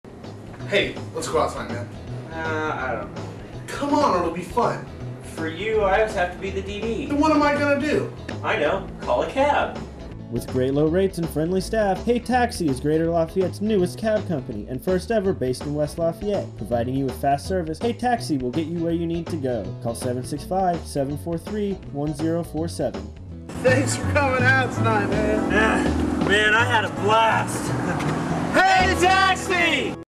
Hey Taxi
Hey Taxi Commercial Filmed in Lafayette.  A taxi company that will pick you up, and take you places.  You just got to shout out it's name.